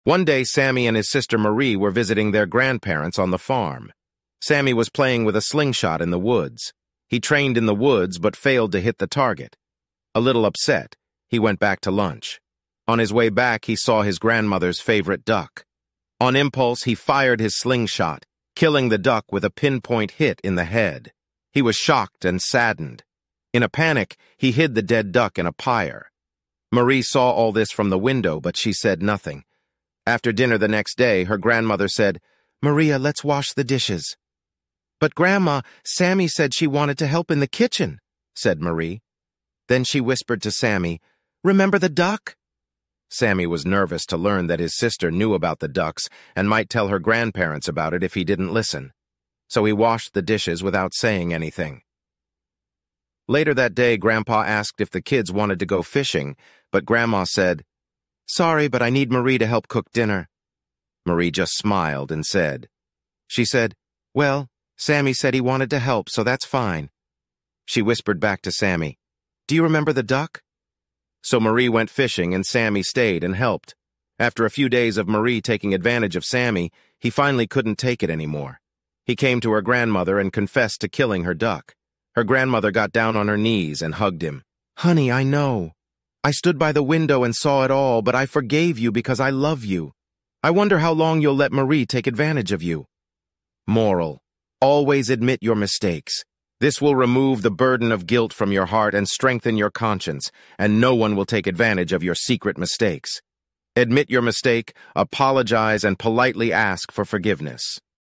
UNLOCKING THE POWER WITHIN: Embracing Mistakes and Seeking Forgiveness
In this heartfelt video, we explore the transformative power of admitting our mistakes and seeking forgiveness. Join us as we delve into a powerful story of guilt, redemption, and the strength of family bonds. Learn how embracing our imperfections can lead to personal growth and liberation. Don't miss this inspiring tale that will touch your heart and leave you with valuable life lessons.